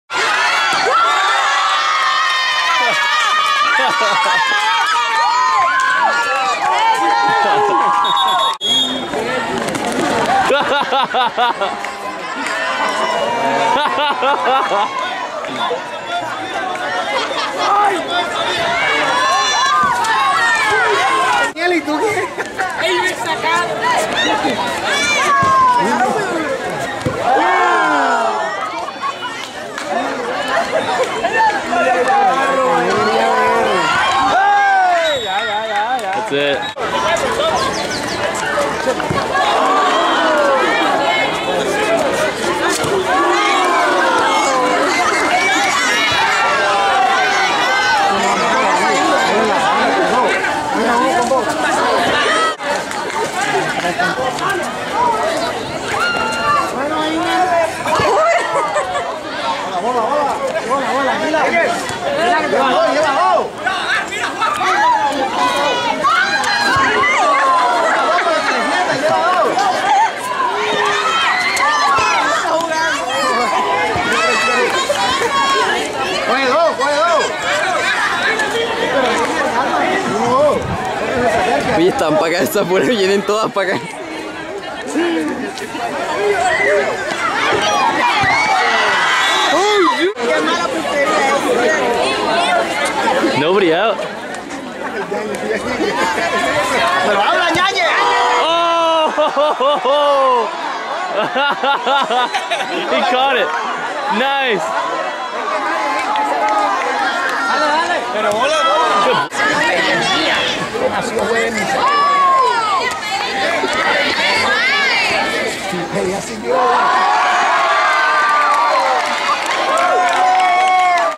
Dodgeball!
La Mina Dodgeball Tournament Highlights. Day One.